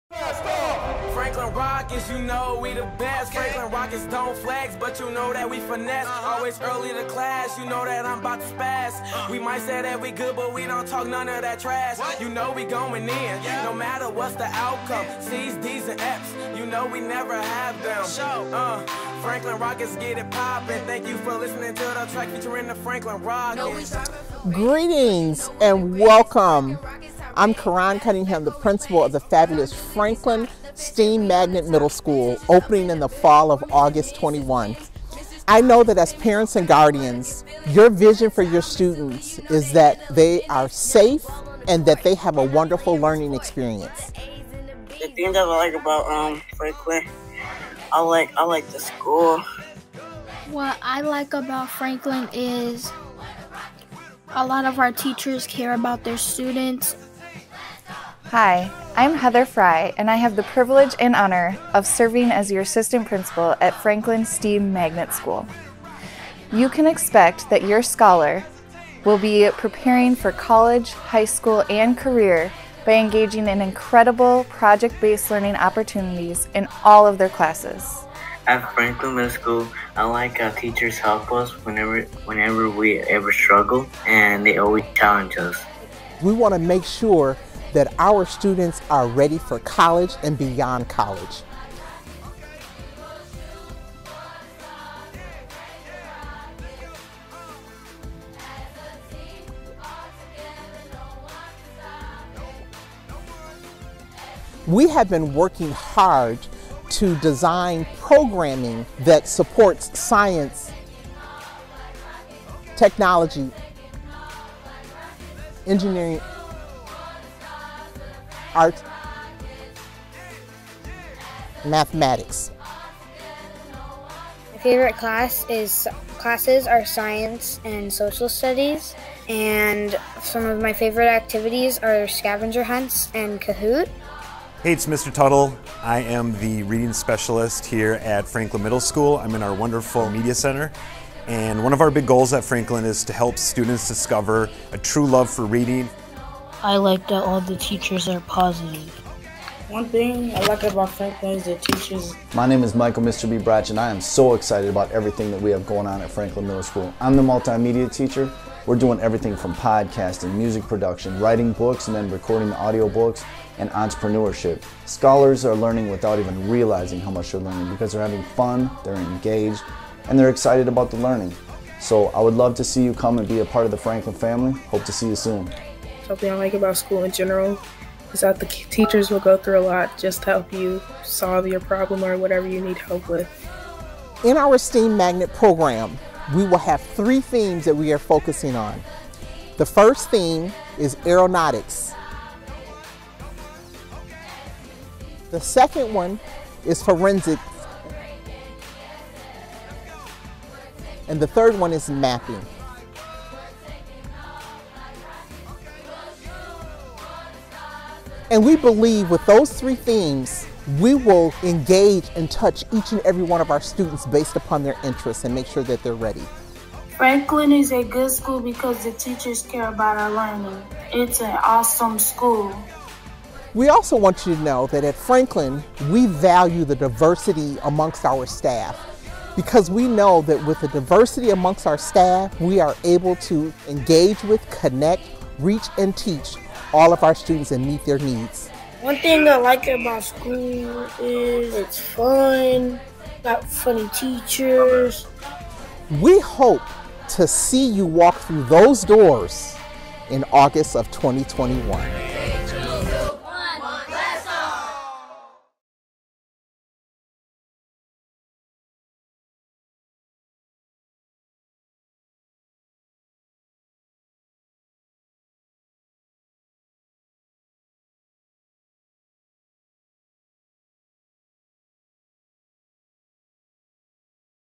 Join us! Franklin STEAM Magnet Virtual School Tour
Global pandemic?  This won't stop us from welcoming you and your family to our school community for an open house.  And it's safe!  We will be doing all of these open houses online this year.

Enrollment for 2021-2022 school year is now open!  Visit ExploreMPS.org to find all the schools Minneapolis Public Schools has to offer.

Inclusive education.  Supportive community.  Lifelong learning.